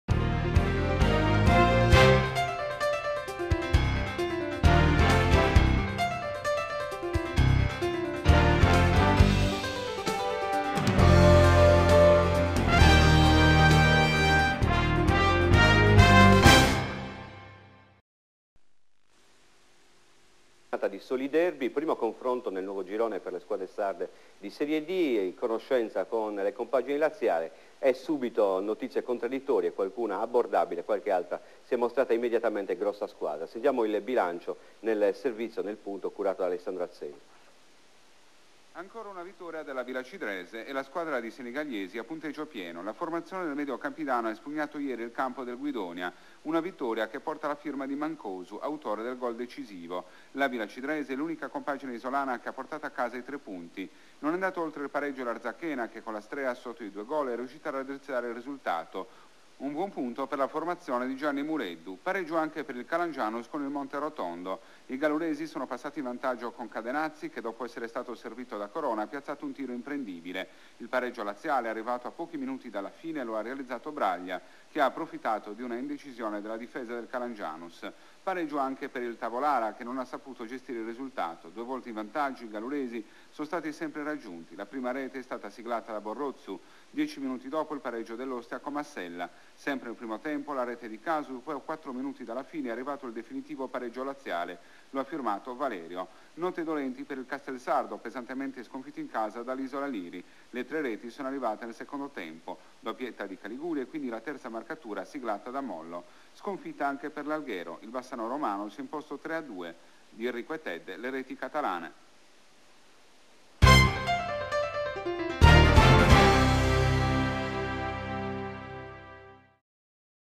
Liritv isola del liri I GOAL DELLA 2° GIORNATA del 9/9/2007
le reti dell'isolaliri a castelsardo del 9 settembre 2007 ed anche le altre realizzazioni